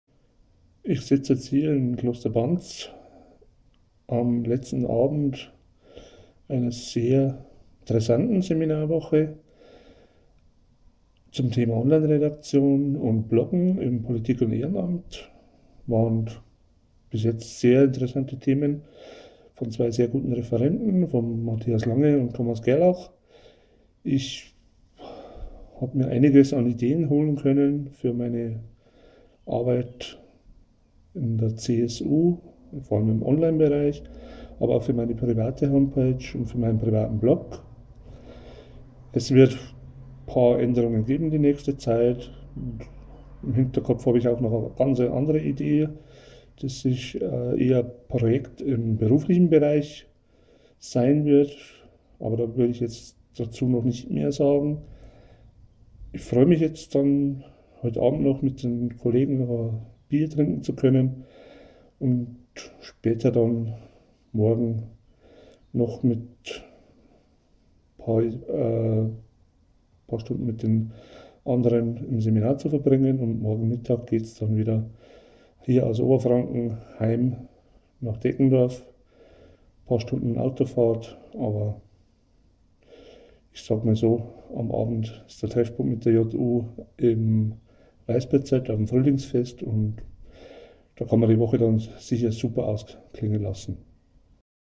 Online-Redaktion und Bloggen - Zwei Seminare in Kloster Banz
Florian Roßmeisl über die Seminarwoche bei der Hanns-Seidel-Stiftung in Kloster Banz über die Themen "Online-Redaktion" und "Bloggen" mit den Referenten Matthias J. Lange (redaktion42) und Thomas Gerlach.